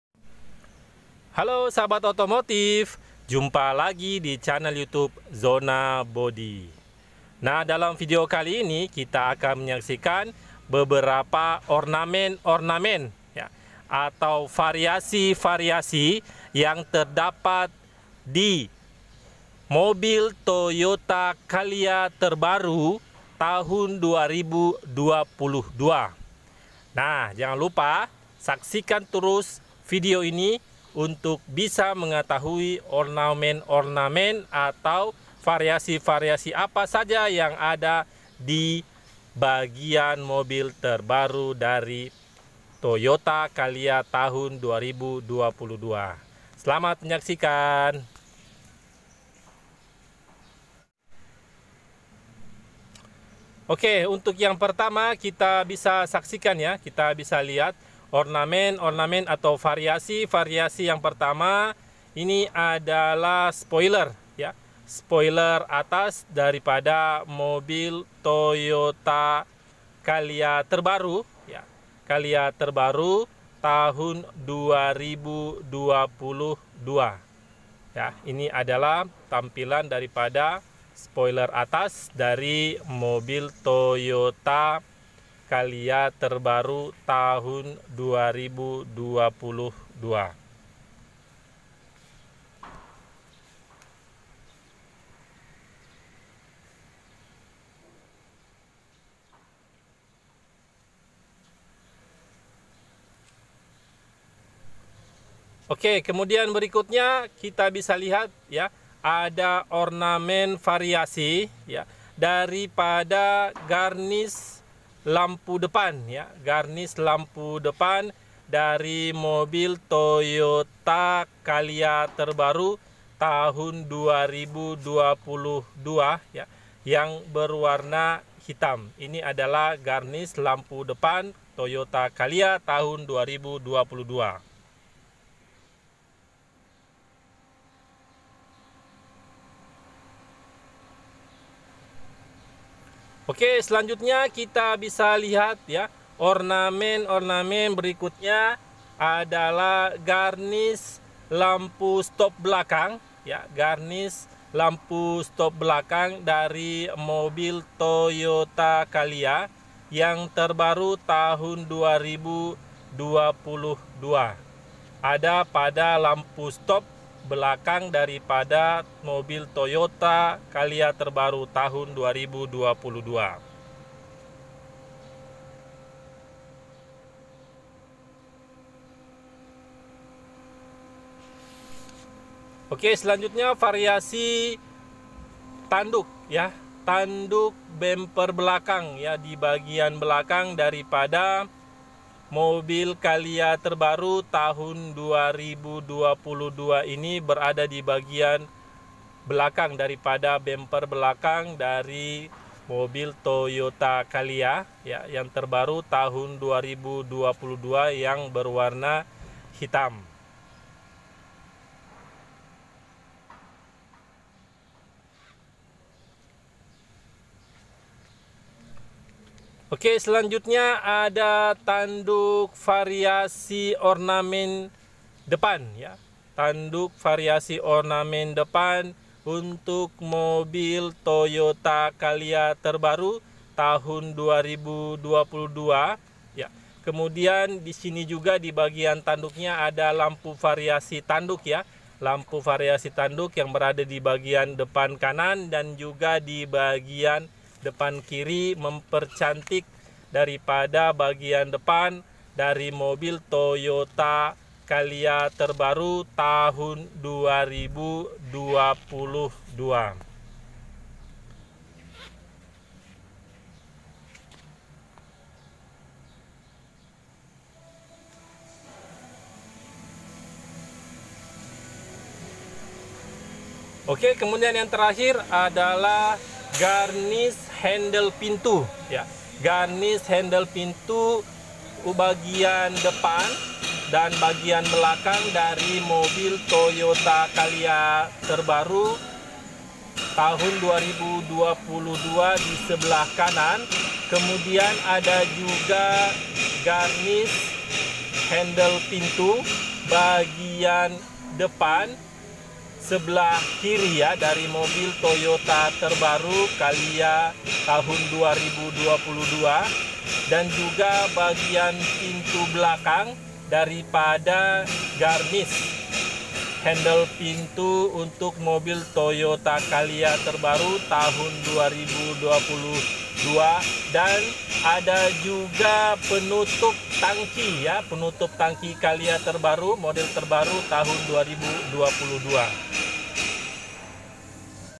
Ornamen dan fariasi untuk mobil Toyota Calya terbaru tahun 2022
begini penampilan ornamen dan fariasi untuk mobil Toyota Calya terbaru tahun 2022 mulai dari spoiler atas, tanduk fariasi depan, tanduk fariasi belakang, garnis lampu depan kiri dan kanan, garnis lampu belakang kiri dan kanan, garnis handel pintu depan belakang kanan dan kiri dan juga garnis tutup tangki untuk ornamen dan fariasinya semua berwarna hitam cocok untuk mobil Calya yang berwarna putih.